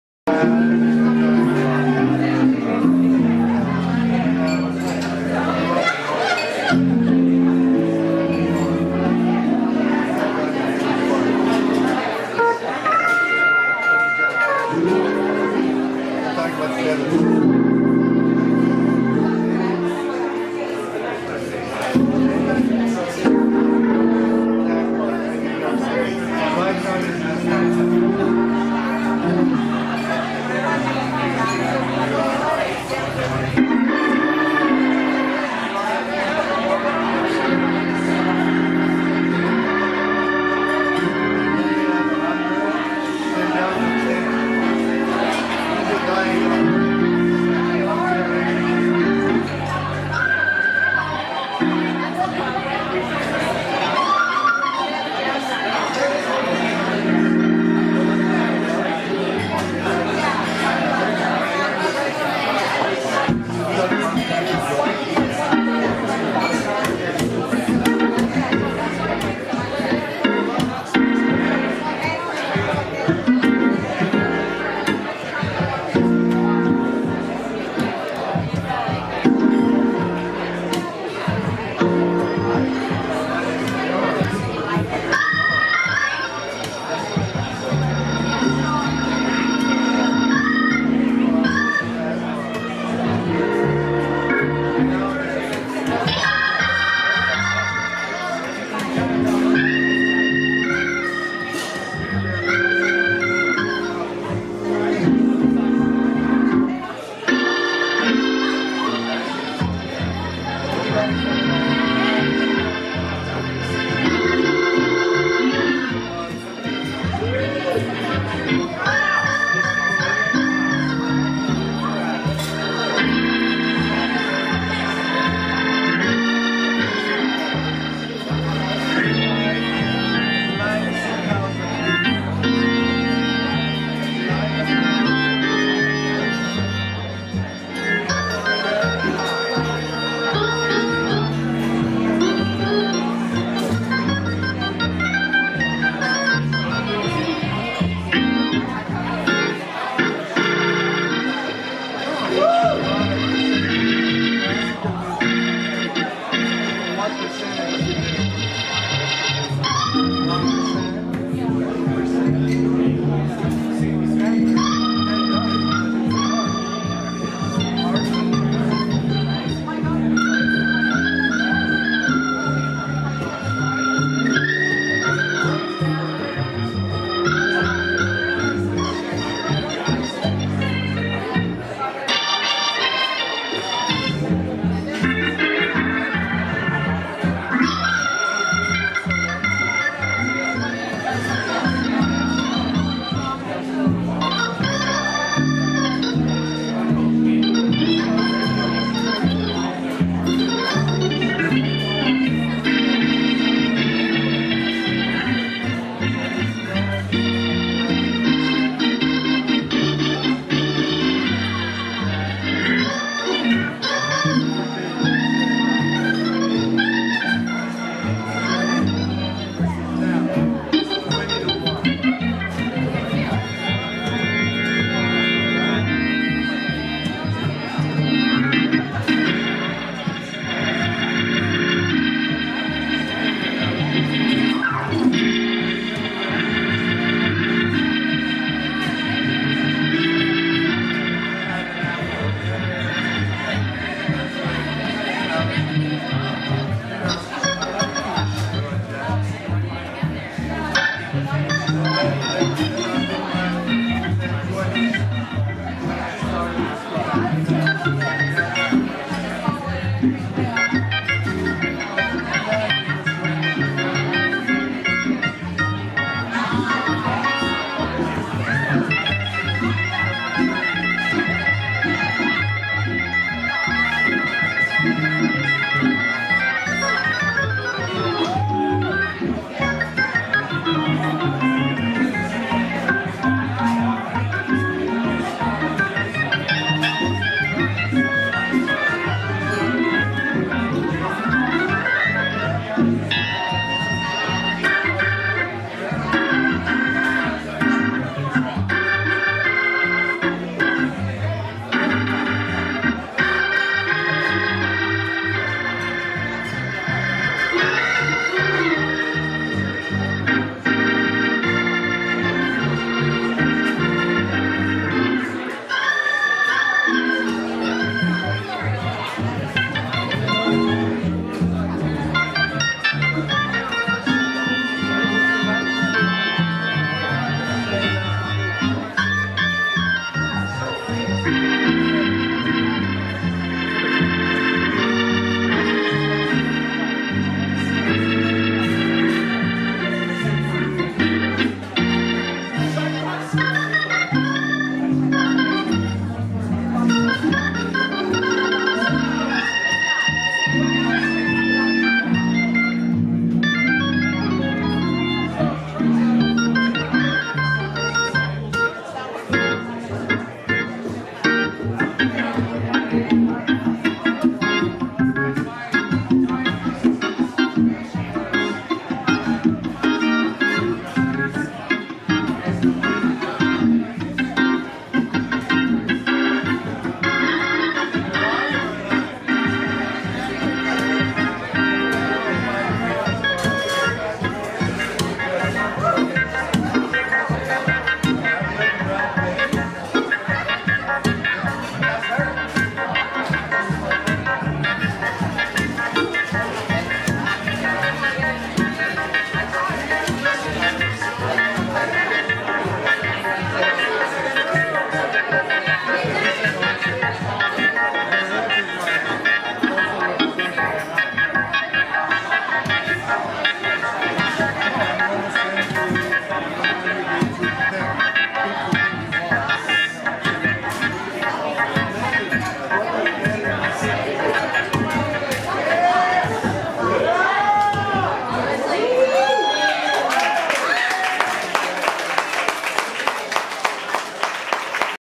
Larry Goldings & David Piltch - The Stowaway (4K)
Larry Goldings and David Piltch open up the Stowaway, a new Piano Bar in DTLA on May 19, 2022.